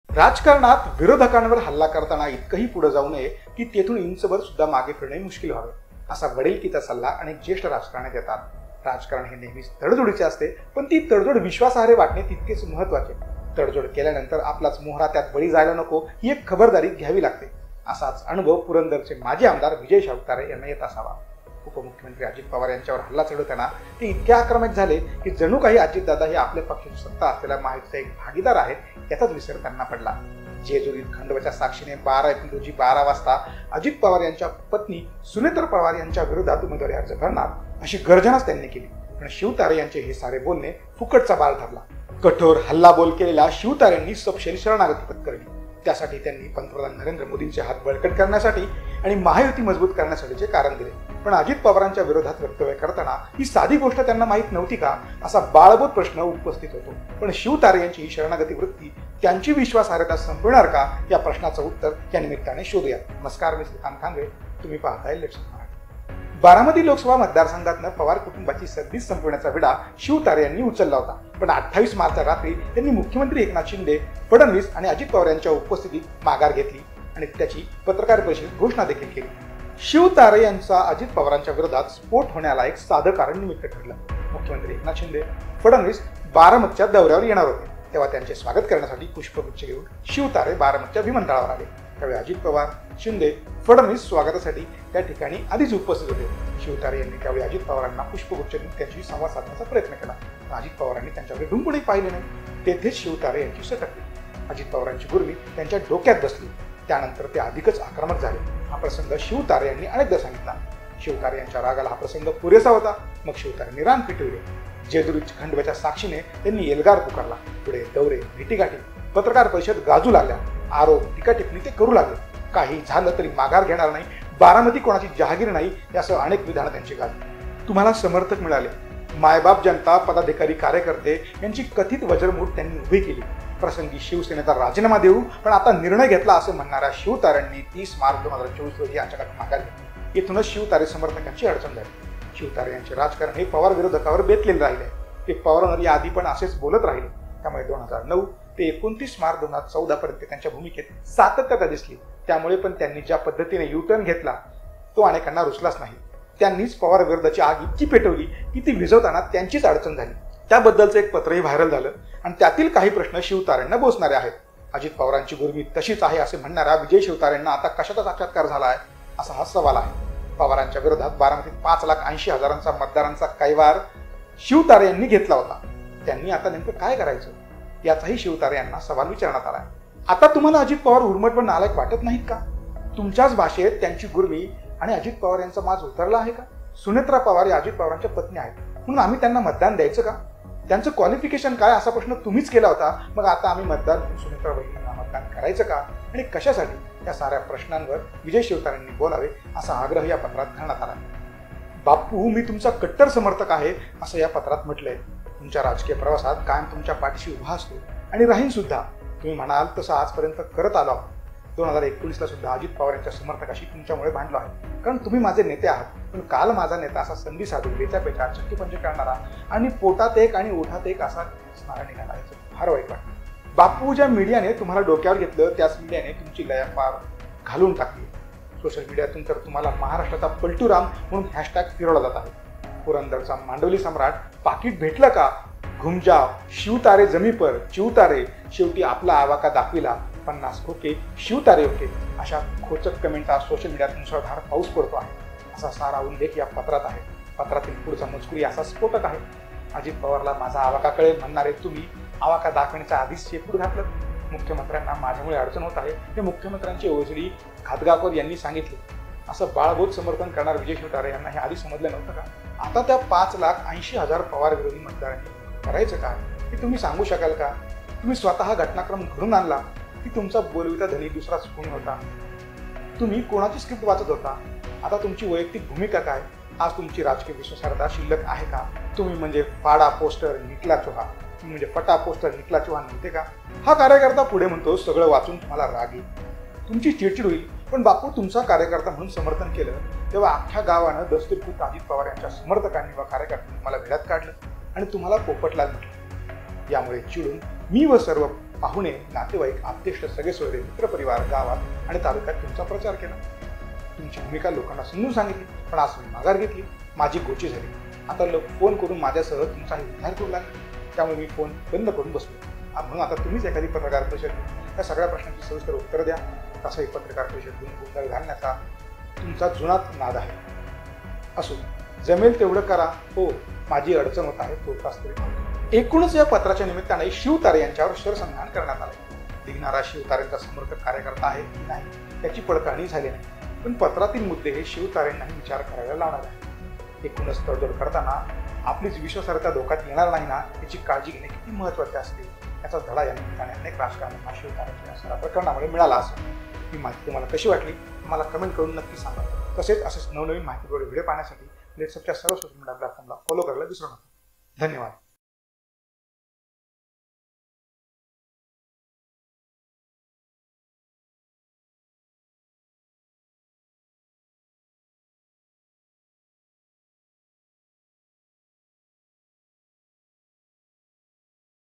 Vijay Shivtare : आधी हल्लाबोल, नंतर शरणागती… शिवतारेंची विश्वासार्हता वाऱ्यावर! | LetsUpp Marathi
बारामती लोकसभेच्या निमित्ताने अजित पवारांच्या विरोधात वक्तव्ये करताना विजय शिवतारेंनी कोणतीही संधी सोडली नाही.मात्र त्यांनी अचानक माघार घेतल्याने त्यांची विश्वासर्हता संपविणार का? या प्रश्नाचे उत्तर यानिमित्ताने शोधूया...